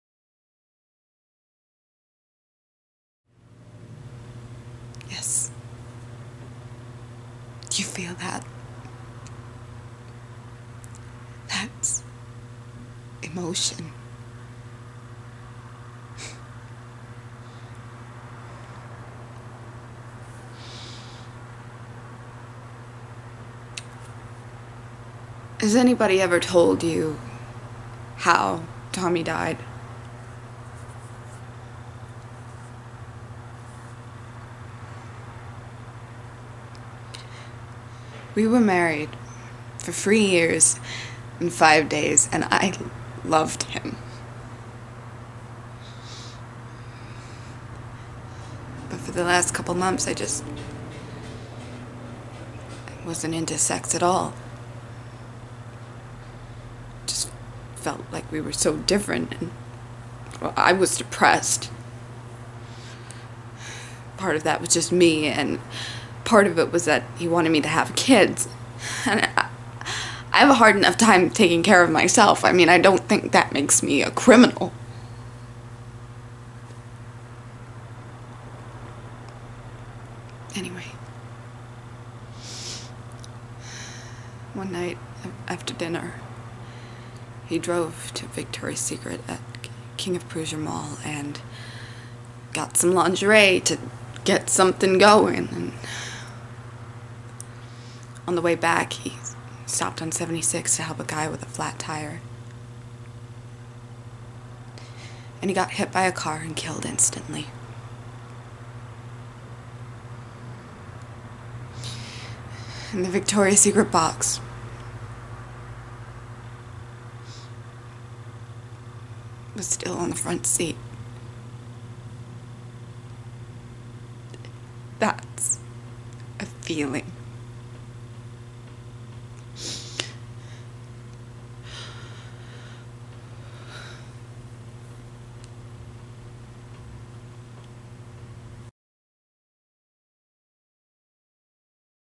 Tiffany Maxwell - Silver Linings Playbook | Monologue
Hello, all! I'm back!

So, I planned to take a month or two out from the acting scene. Those one or two months have lasted for 18 months, now, and I feel so behind. I have wanted to get back into acting, but as I started working full-time, I found the idea of juggling auditions, rehearsals, shows and shoots impossible with my office hours. I was always so tired after work, so I just let the drudges of daily, ordinary life get the better of me and my time, unfortunately.

However, it's time to get back to dream chasing, and working hard at it, too. I feel so rusty and new to the scene, despite how many years I was involved in film and theatre beforehand, so I thought I'd go back to where I started - uploading monolgoues to YouTube and asking for your feedback. All comments welcome. Please feel free to tell me what you think, and any ideas on how I can improve; likes, dislikes etc.

I haven’t seen this film in years, and didn’t want to watch this scene again so I could have a fresh take on the monologue itself. I’m no Jennifer Lawrence, but I think this character is open for different interpretations. What do you think?

I would really appreciate your help.

A x